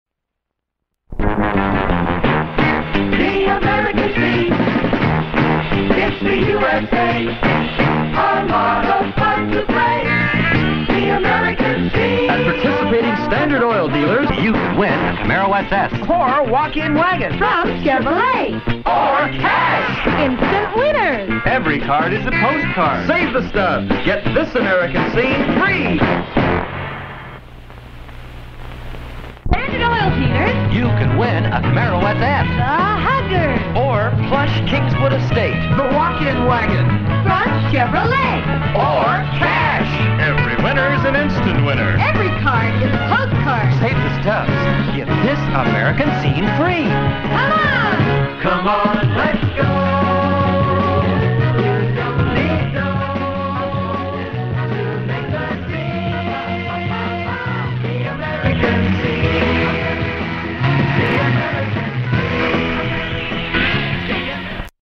Amoco/Standard Oil "American Scene" commercials 1969 (2 spots)
Two really groovy youth-orientated late 60s commercials promoting a contest for Standard Oil in the Midwest, also known as American Oil outside the Midwest, later Amoco, then bp. Dig the chick riding the revolving Standard sign in the second spot!